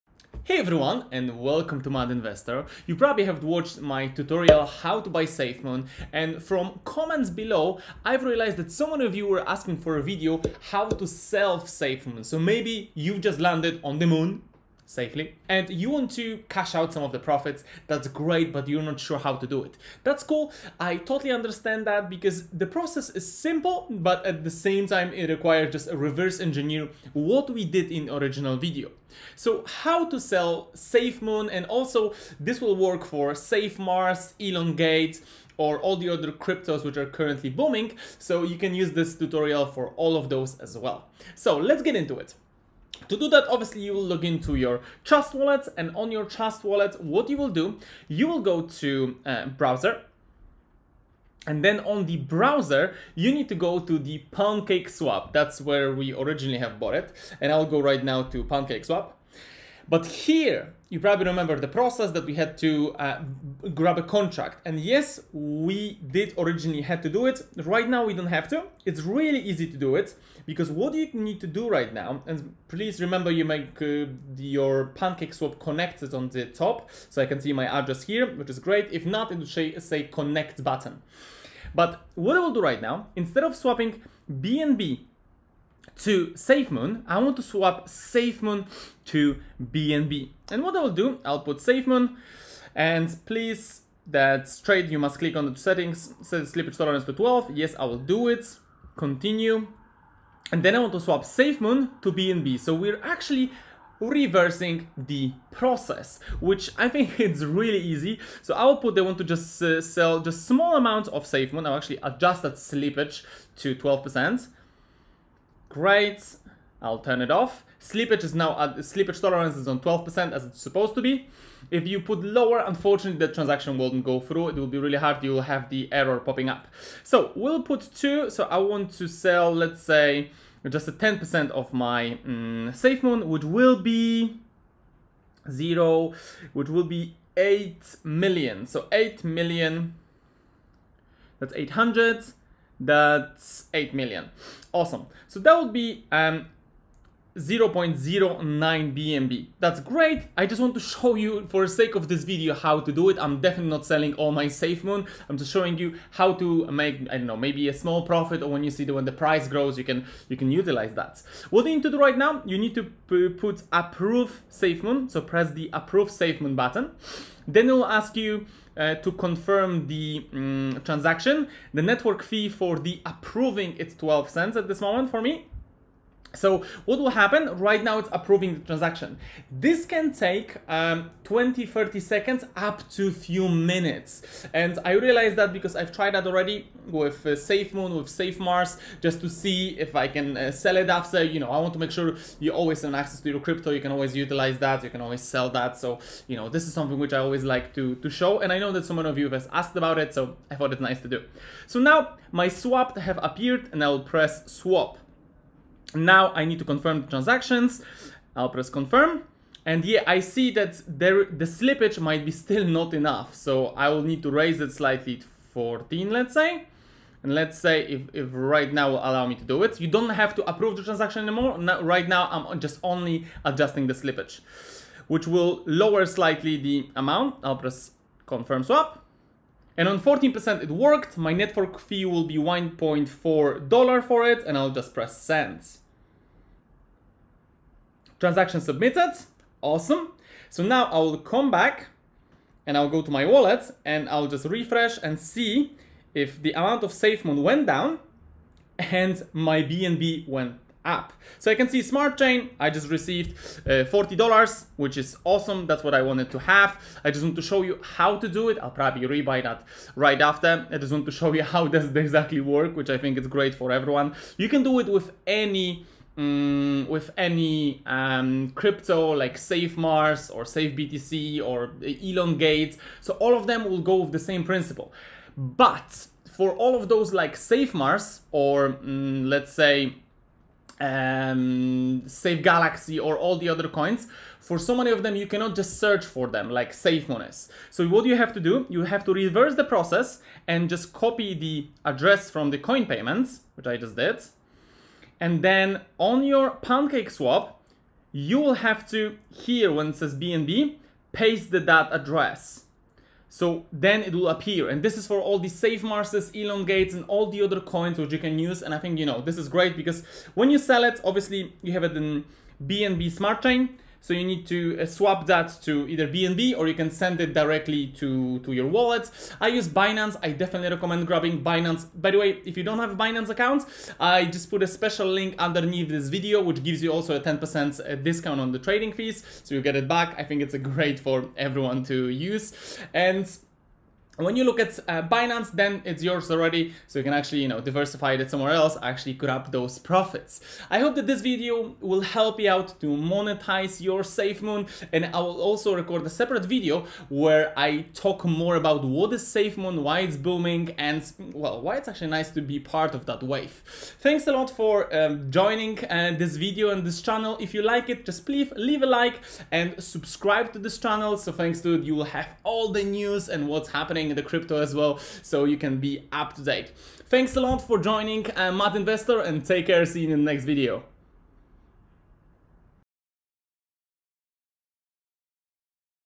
How To SELL Safemoon Coin via PancakeSwap Tutorial - The EASIEST Way Using Trust Wallet!
How To Sell Safe Moon Coin on Trust Wallet via Pancake Swap. You can sell same way Safemars, Elongate, safesun, safetesla and others
For Android and iPhone Users.

👇 Don't have binance account? Register here for extra perks

Step 1: Go to your Trust Wallet, Press Dapps

Step 2: Open PancakeSwap

Step 3: Choose Safemoon or paste safemoon contract address instead of bnb

Step 4: Tick “I understand” on slippage amount and press Continue

Step 5: Setup your slippage amount at around 12-14%

Step 6: Choose “To” and select bnb

Step 7: Press Approve Safemoon, confirm the approval, and wait few seconds

Step 8: Press “Swap” and confirm the transaction

Step 9: You’ve successfully swapped safemoon to bnb

🛍 Our clothing store! 🛍